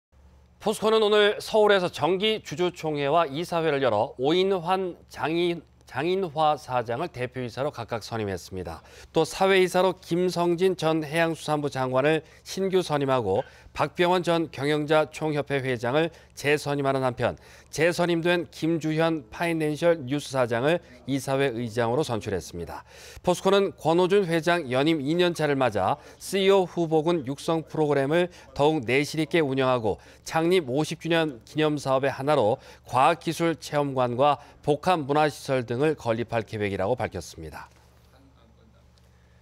[포항MBC뉴스]포스코 주총.. 김성진 전 장관 사외이사 선임
포스코는 오늘(9일) 서울에서 정기 주주총회와
이사회를 열어 오인환, 장인화 사장을
대표이사로 각각 선임했습니다.

또 사외이사로 김성진 전 해양수산부 장관을
신규 선임하고, 박병원 전 경영자총협회 회장을
재선임하는 한편, 재선임된 김주현
파이낸셜뉴스 사장을 이사회 의장으로
선출했습니다.

포스코는 권오준 회장 연임 2년차를 맞아
'CEO 후보군 육성 프로그램'을 더욱 내실있게 운영하고, 창립 50주년 기념사업의 하나로
과학기술체험관과 복합문화시설 등을 건립할
계획이라고 밝혔습니다.